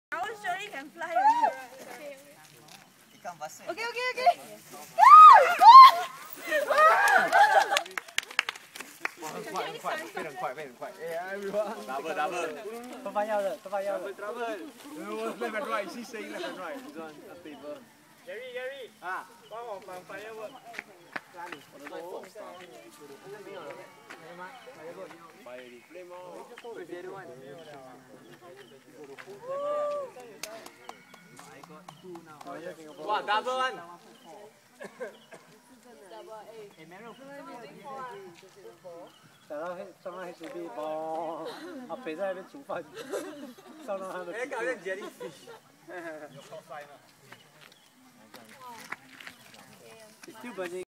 Launching Kongming lanterns during lunar eclipse (double trouble)
In a school at Pai